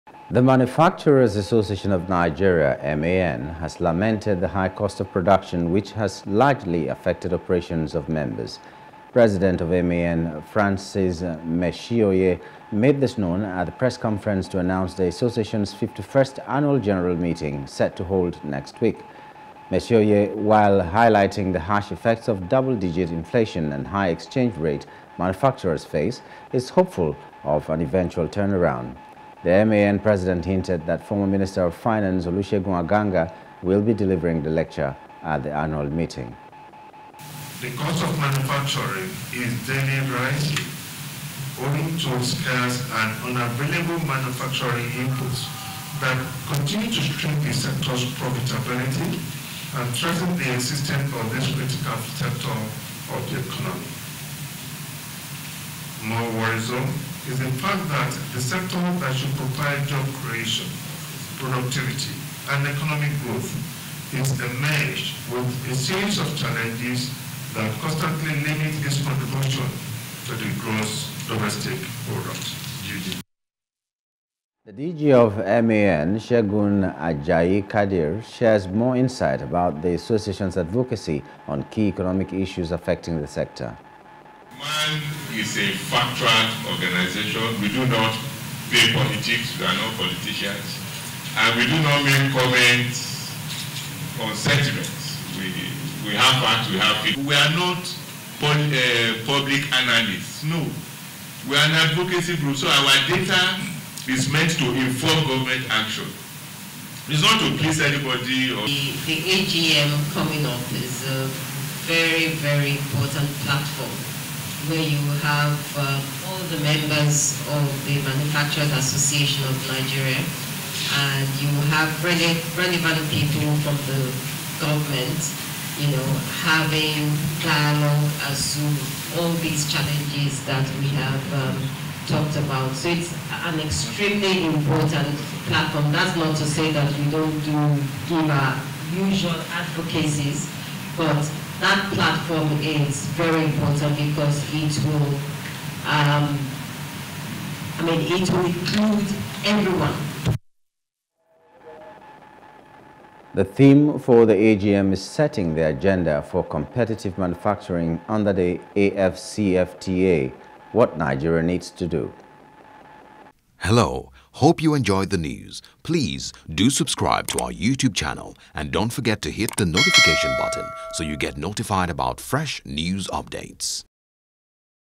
Manufacturing: M .A .N Laments High Operations Cost, Set To Hold Annual Meeting
The manufacturers Association of Nigeria, M.A.N. has lamented the high cost of production which has largely affected operations of members.